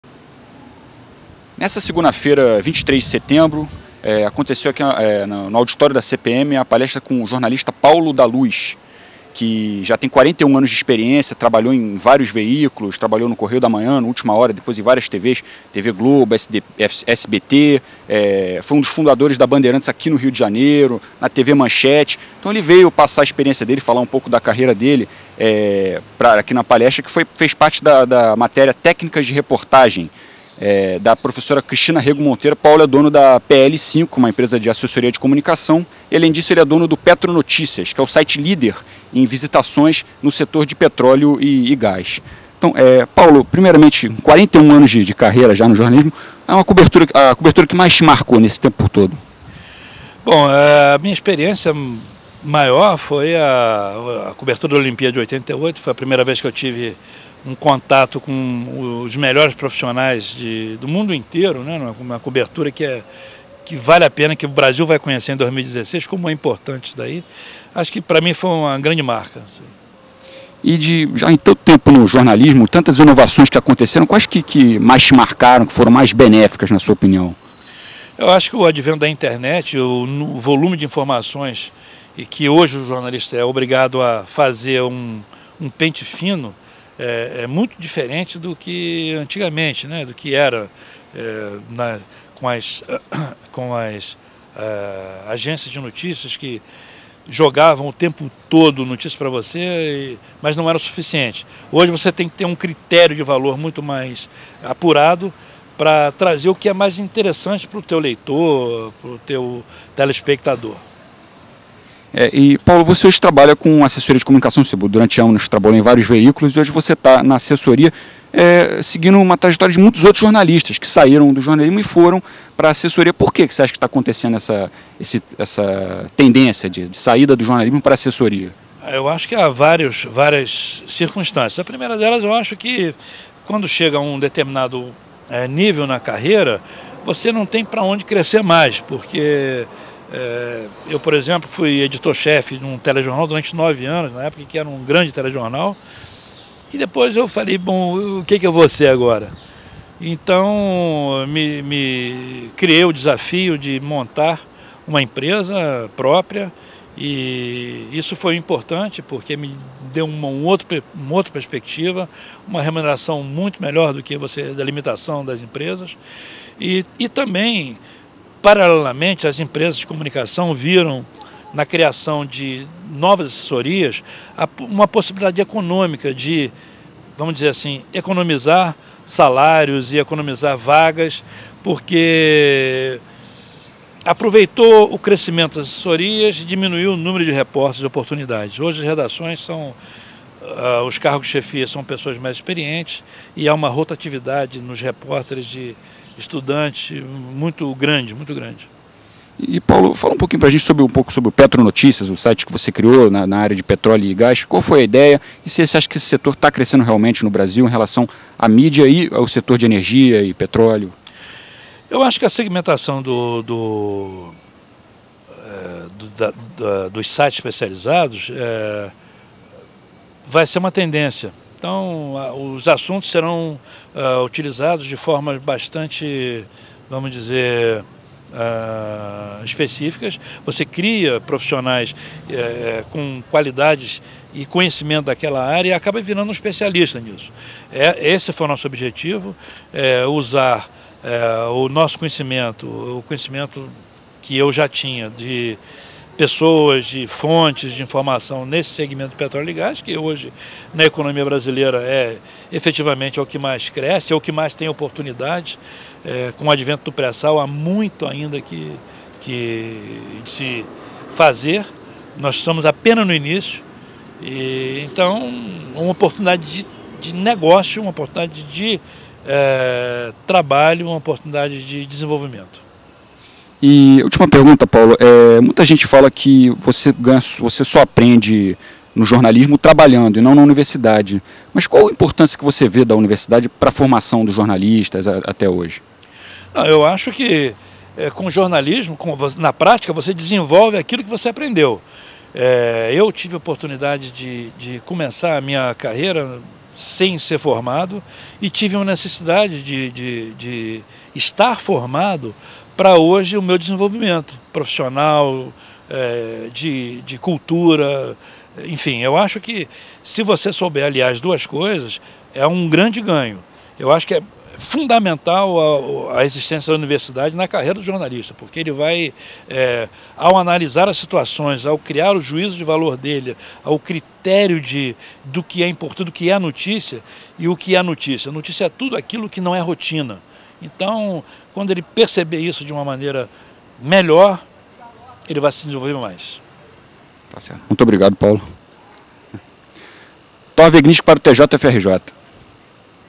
Jornalista Paulo da Luz realiza palestra no auditório da CPM
Palestra ocorreu na segunda-feira, 23 de setembro de 2013.
Com 41 anos de experiência, Luz trabalhou em vários veículos de comunicação, como TV Globo, SBT, Bandeirantes e Manchete. Atualmente, é dono da PL5, empresa de assessoria de comunicação, e do Petronotícias, site líder de acessos na área de petróleo e gás. Entre os assuntos, o convidado falou da cobertura que mais marcou sua carreira e das inovações mais importantes no jornalismo. A palestra fez parte da disciplina Técnicas de Reportagem, da professora Cristina Rego Monteiro.

Repórter=Thor Weglinski
Câmera=Daniel Araújo